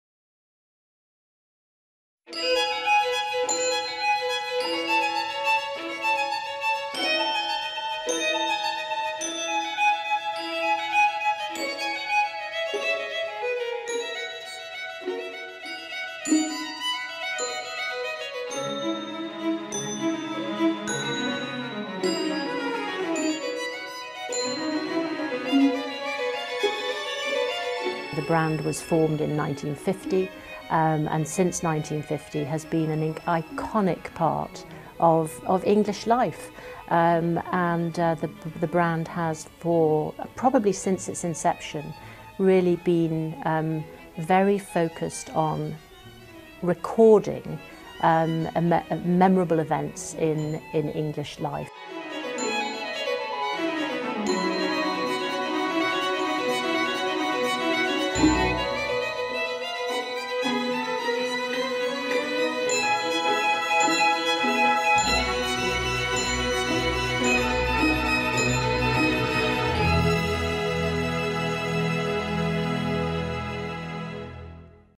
Royal ceramics to celebrate the wedding between Prince Harry and Meghan Markle | euronews Living
Traditional artisans are working on a number of commemorative collections for the royal wedding.…